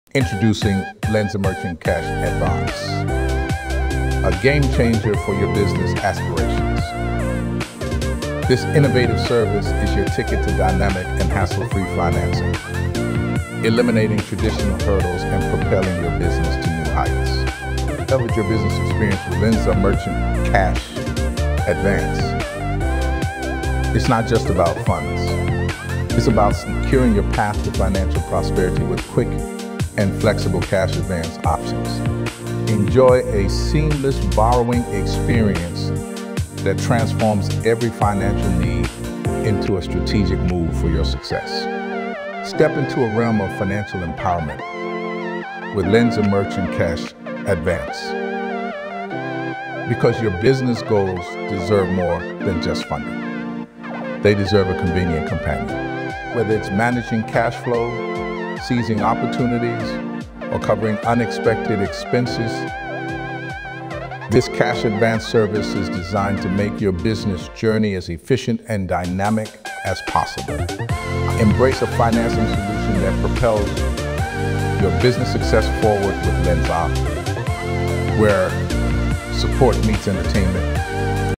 Lendza Merchant Cash Advance Review! Borrow Up To $350,000
Get working capital now and use your future earnings to pay it off.

A merchant loan advance isn’t a loan at all. It’s a way to borrow against your future earnings. More commonly known as a Merchant Cash Advance (MCA), this type of financing is typically for those who cannot receive a bank loan. With an MCA, you get your money relatively quickly, and then you pay it back over the next several months through a portion of your credit card sales.

Merchant Cash Advance at a Glance
The first question you should ask yourself before taking out a merchant cash advance is whether or not you’ll be able to pay it back with the added fees. Factor rates on merchant cash advances can be high. Here are the main points:

The maximum funding amount for a merchant cash advance will be determined by your provider.
Your merchant cash advance will have a factor rate instead of an interest rate. Your factor rate could be anywhere from 1.14 to 1.48 or higher.
The funding term for a merchant cash advance is a little different than with other types of funding. Since you are borrowing against your future earnings (and since you pay it back as you earn money) the term is determined by how long it takes you to earn enough money to pay back the funding and the fees. On average, this takes around eight to nine months.

Maximum Funding Amount
Up to $200,000

Funding Term
Around nine months

Factor Rates
1.14 to 1.48

Time to Funds
In as little as one business day

It’s not always about how you made your money it’s more about how you spend the money that will determine your legacy!!!

Making the money is easy keeping it is the hard part!